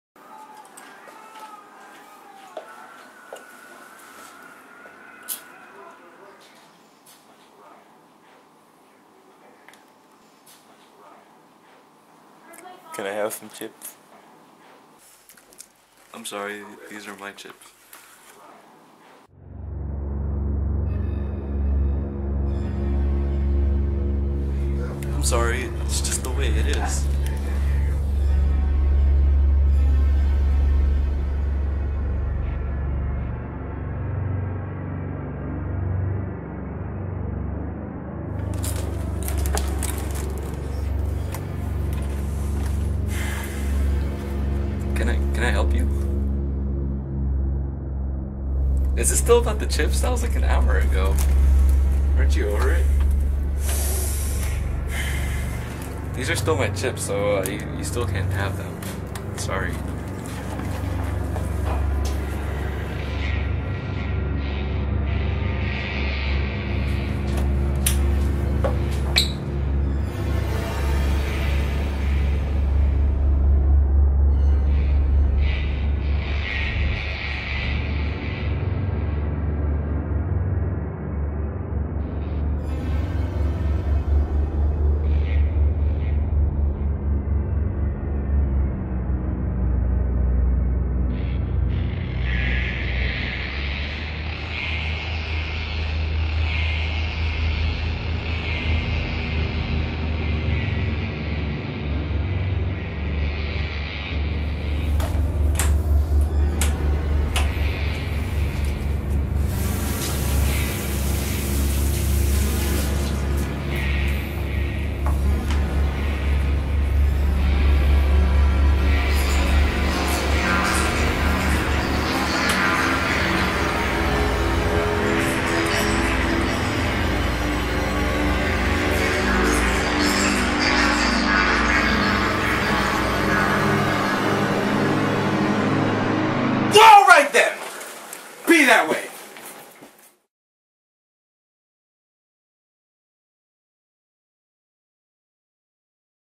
Lewis wants chips
And he won't leave until he gets them... maybe.